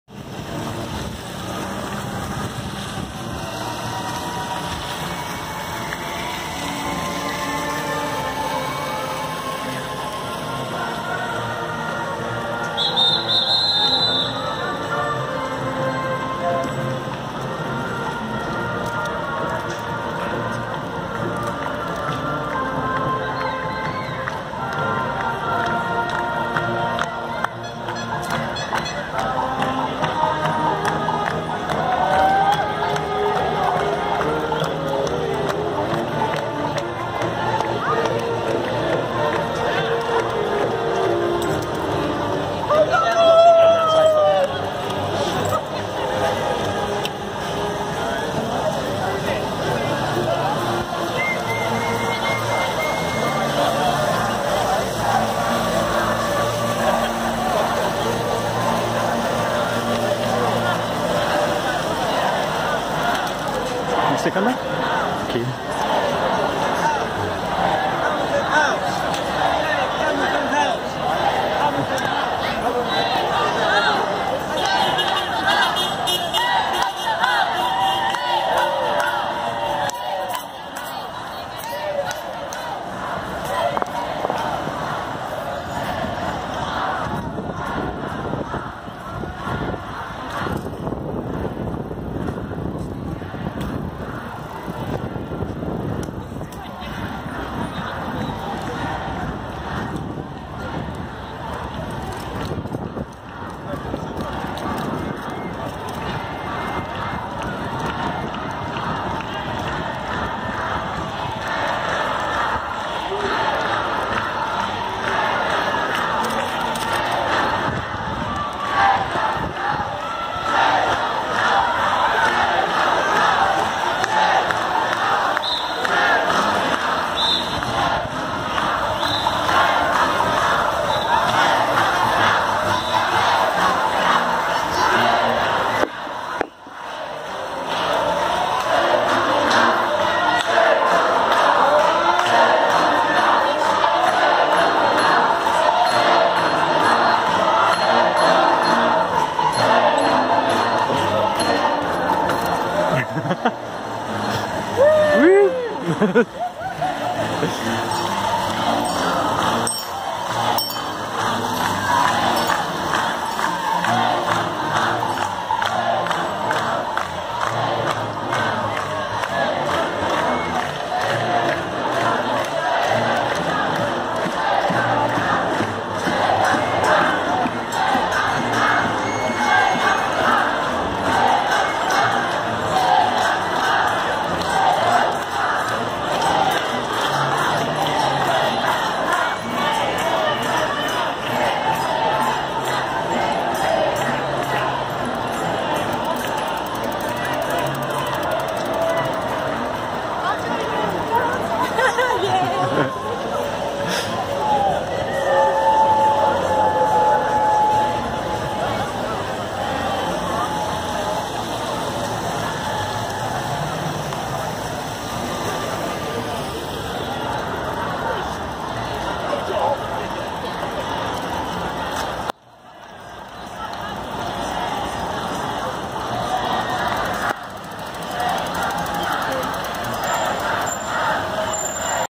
Save Hamilton House Demo
- A cycle round the last bits of a march, part of a demonstration hoped to save Hamilton House, a valuable arts and community space in Bristol's Stokes Croft.